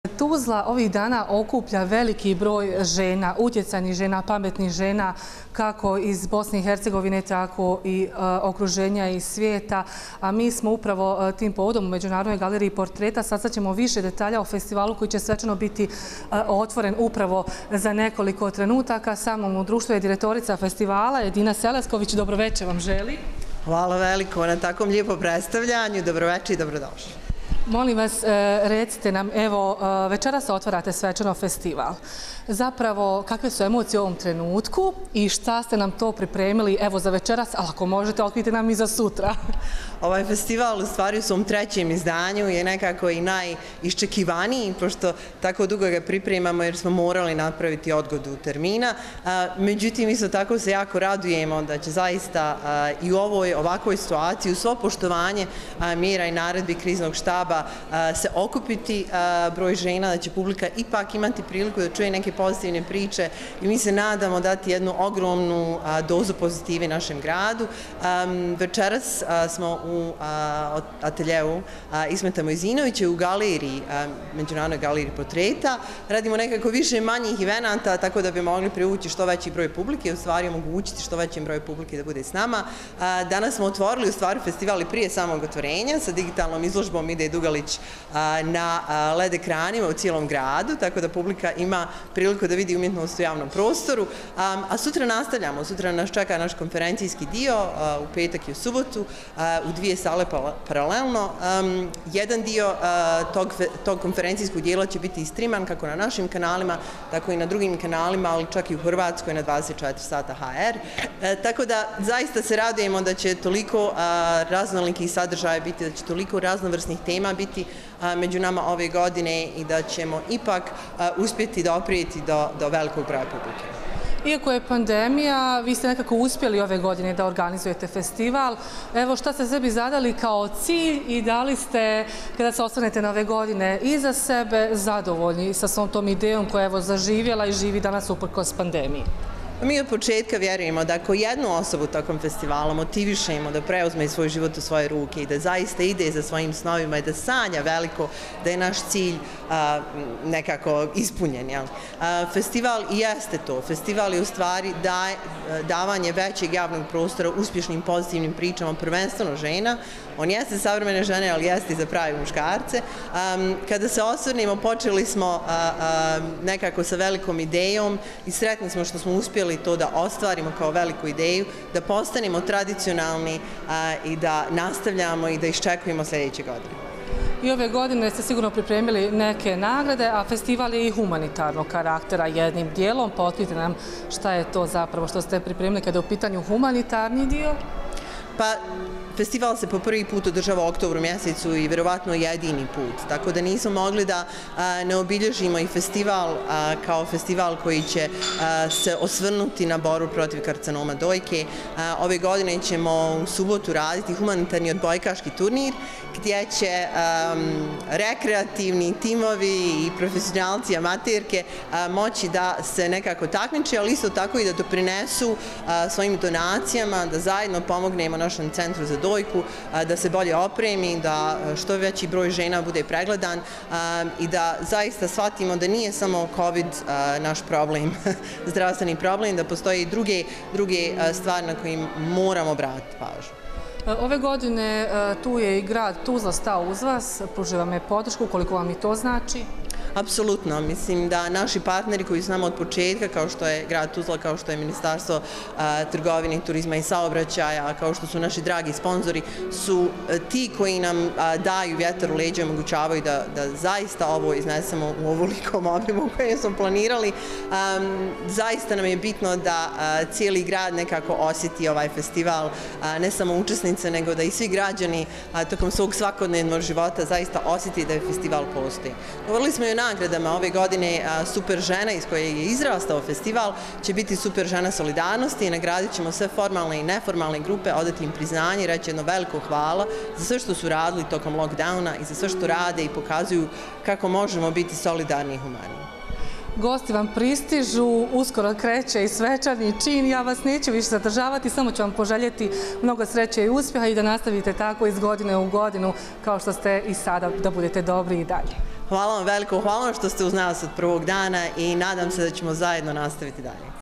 RTV7 uživo pred početak Festivala savremene žene
U Tuzli je večeras počeo Festival savremene žene koji je kreiran kao platforma otvorenog dijaloga čiji alati su odlučni i inspirativni govori najuspješnijih dama regije. Kroz teme života od politike i ekonomije preko kulture, medija, biznisa do blogerki, feng shui-a slavi se uspjeh i motiviše. Iz organizacionog odbora poručuju - Spremite se da zajedno stvorimo najljepše uspomene.